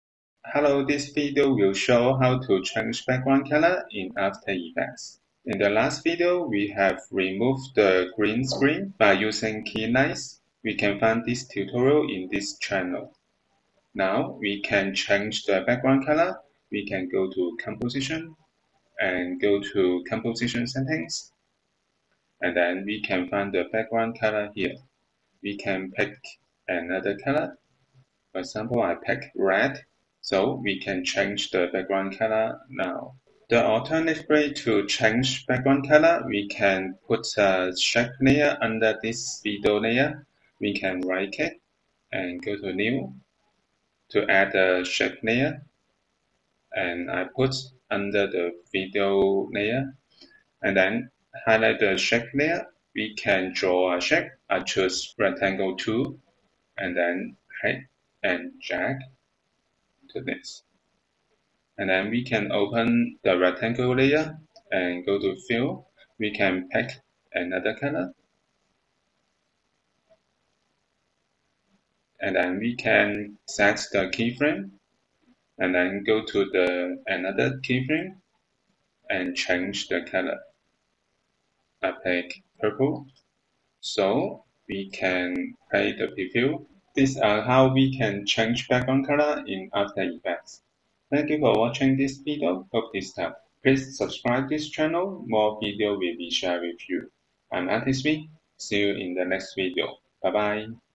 After Effects How to change background color | AE tutorial for beginners
After Effects Basics Tutorial to show how to change background color. In this video, we will go through how to change background color in AE, and we can set keyframe to change the background color using shape layer as a background. Hope this helps.

Timestamp
0:00 Intro – How to change background color in After Effects
0:20 Change Composition Background Color
0:38 Add Shape layer with color as background
1:03 Set frame to change background color with time